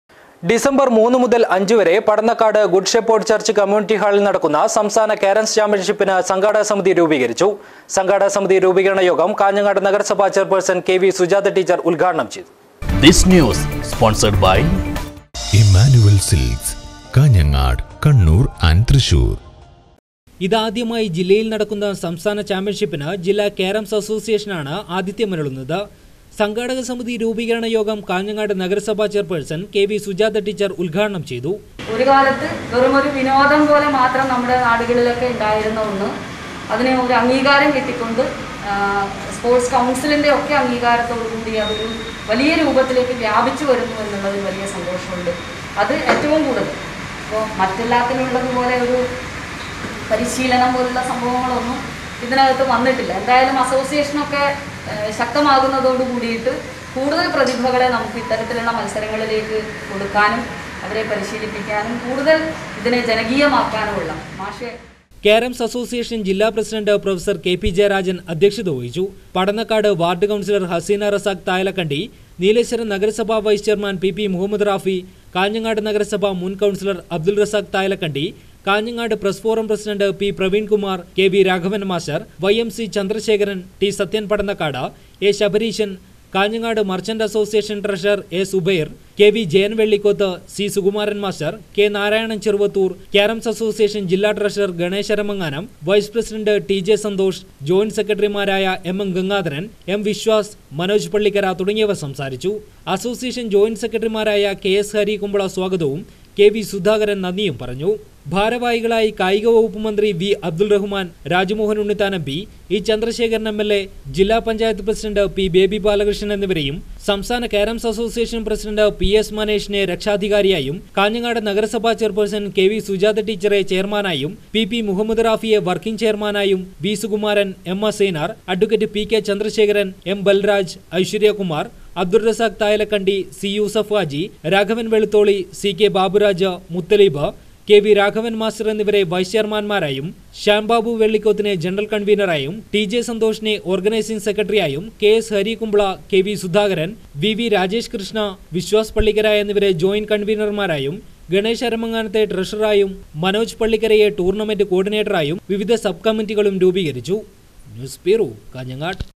സംസ്ഥാന കാരംസ് ചാമ്പ്യന്‍ഷിപ്പിന് സംഘാടക സമിതി രൂപീകരിച്ചു.
ഡിസംബര്‍ 3 മുതല്‍ 5വരെ പടന്നക്കാട് ഗുഡ് ഷെപേഡ് ചര്‍ച്ച് കമ്മ്യൂണിറ്റി ഹാളില്‍ നടക്കുന്ന സംസ്ഥാന കാരംസ് ചാമ്പ്യന്‍ഷിപ്പിന് സംഘാടക സമിതി രൂപീകരിച്ചു. സംഘാടക സമിതി രൂപീകരണ യോഗം കാഞ്ഞങ്ങാട് നഗരസഭാ ചെയര്‍പേഴ്സണ്‍ കെ.വി.സുജാത ഉദ്ഘാടനം ചെയ്തു.